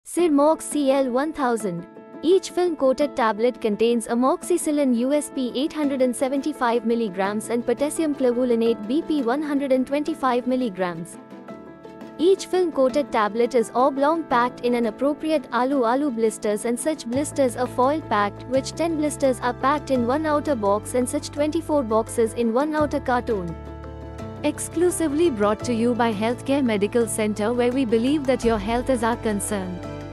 Sirmox CL 1000
Each film coated tablet is oblong  packed in an appropriate Alu Alu Blisters and such blisters are foil packed which 10 blisters are packed in one outer box and such 24 boxes in one outer carton.